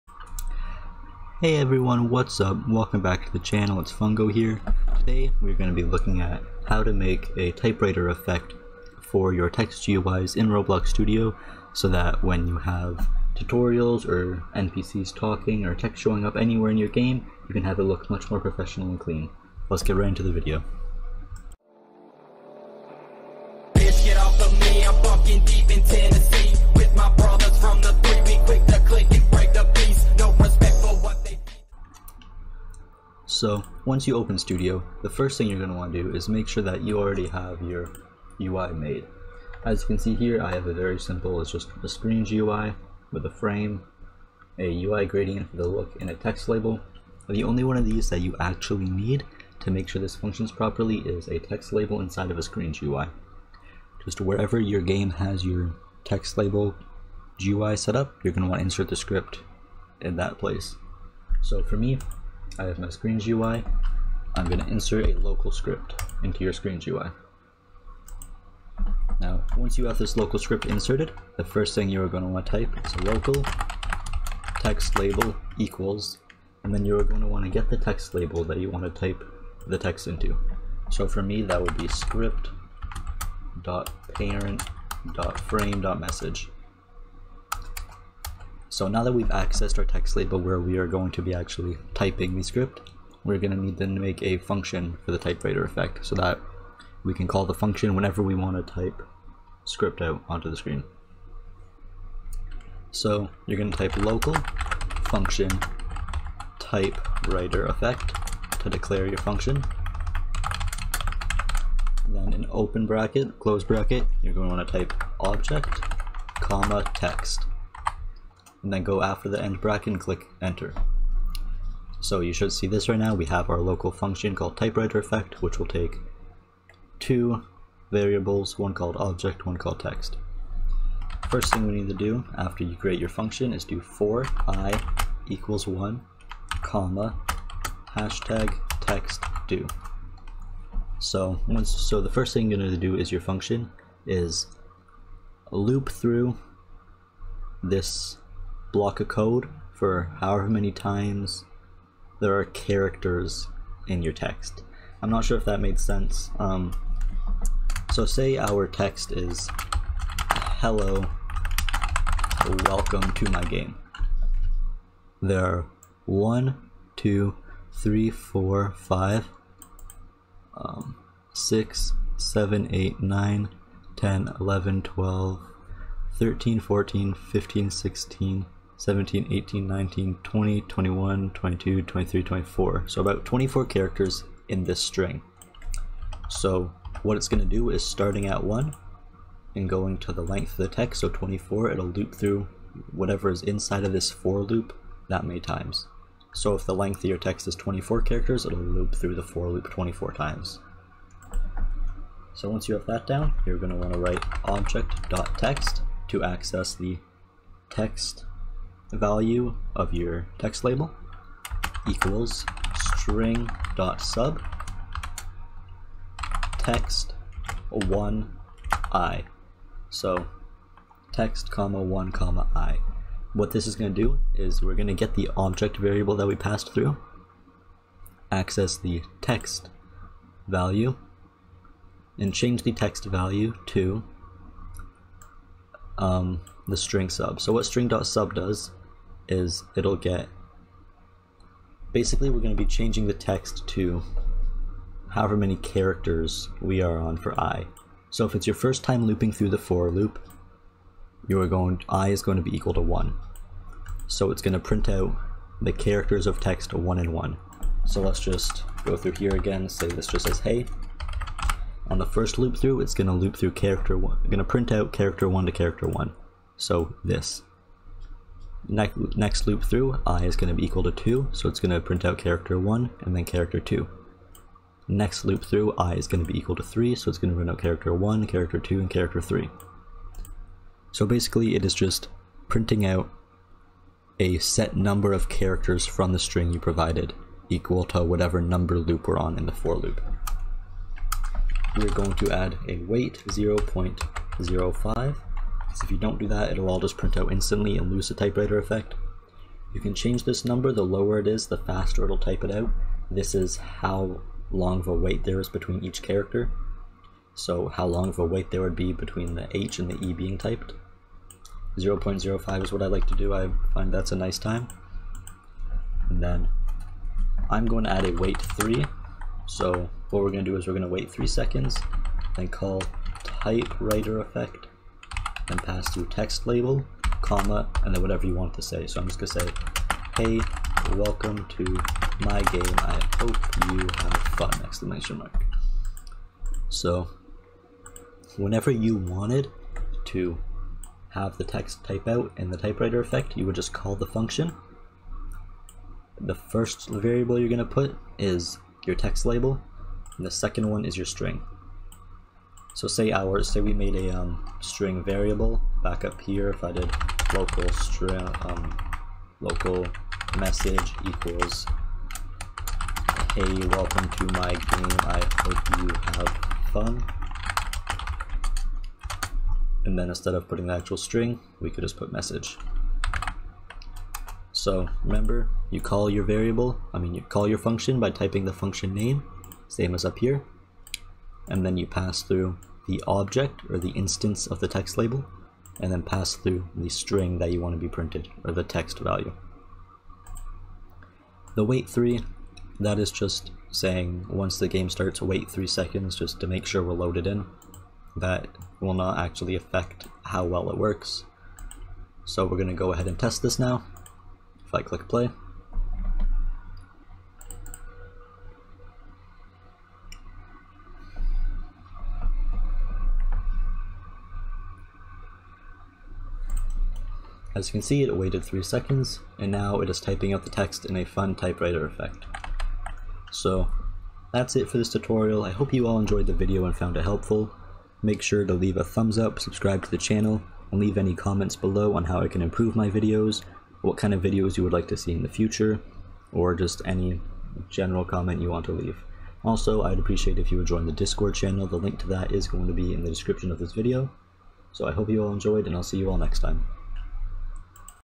How To Add A Type Writer Effect | ROBLOX Studio
Today I will be showing you how to add a typewriter effect on to your text labels in ROBLOX studio to make your games look more clean and professional!